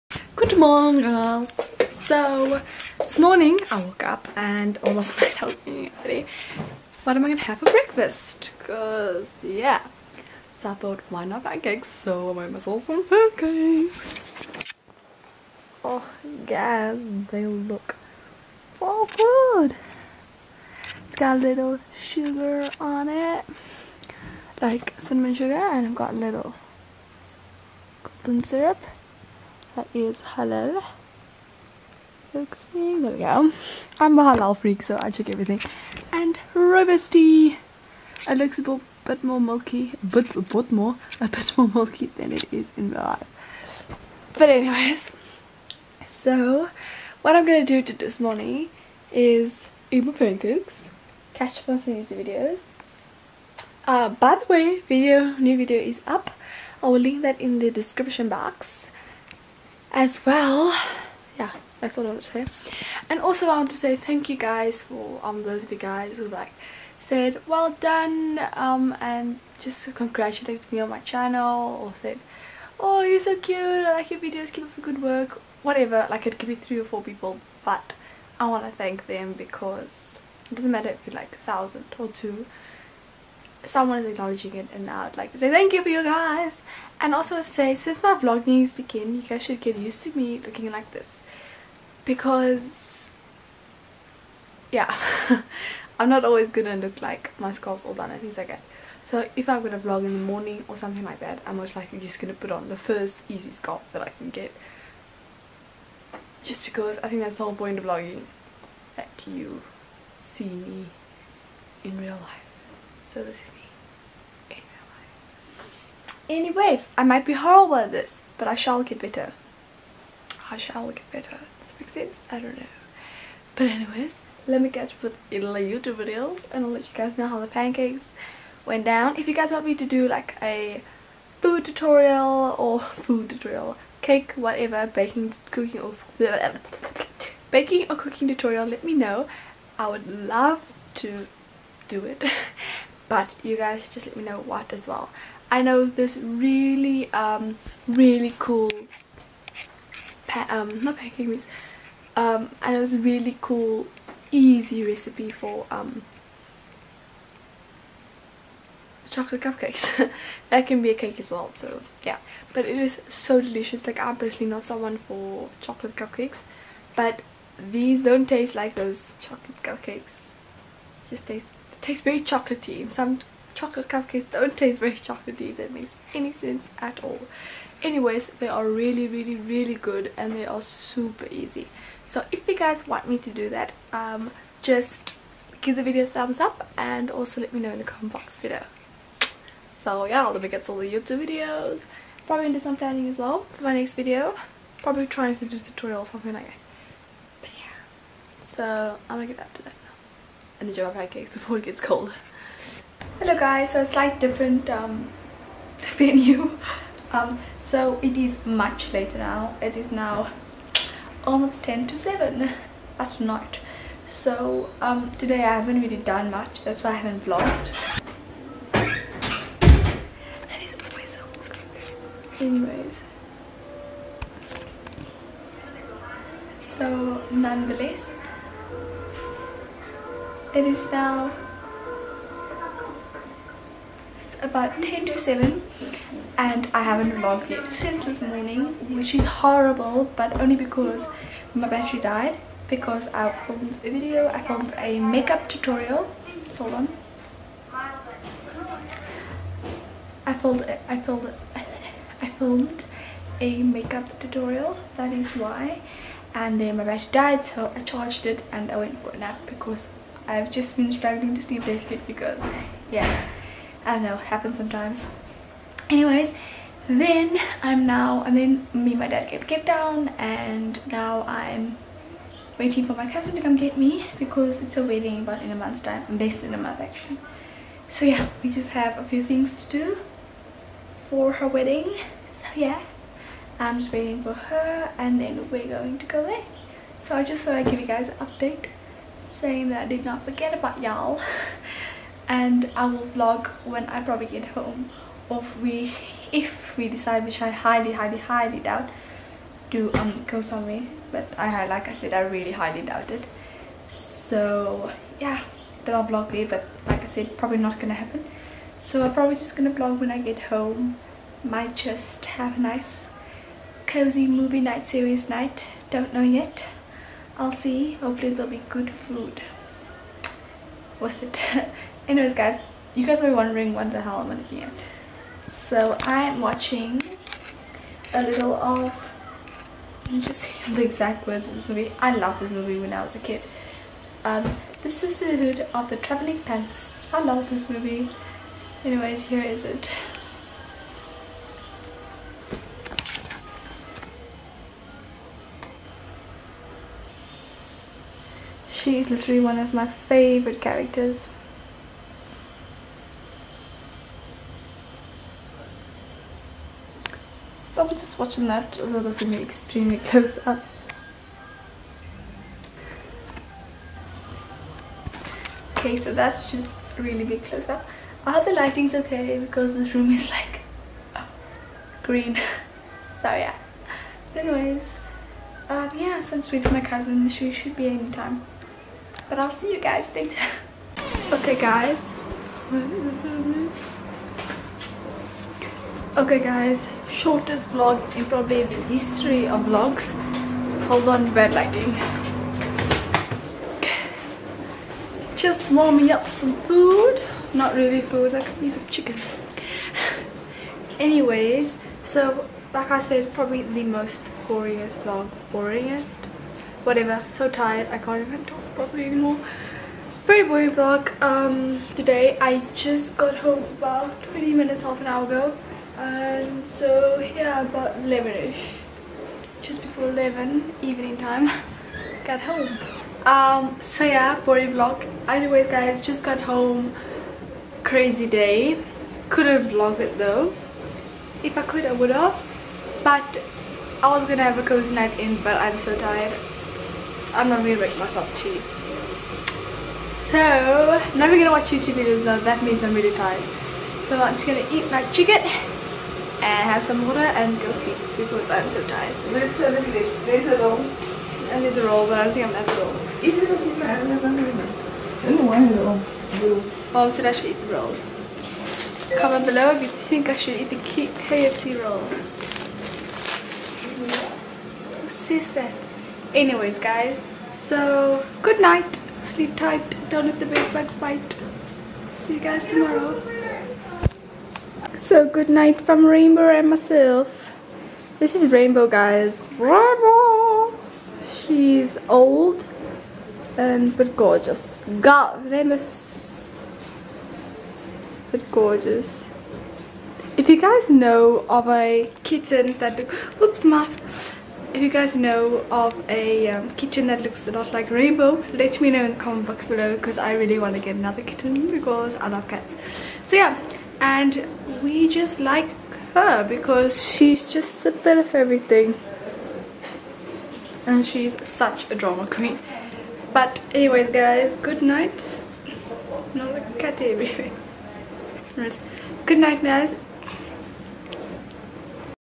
Pancakes for breakfast! | Aqeelah Hendrickse
First weekend vlog, sorry that its a bit all the place, still pretty new.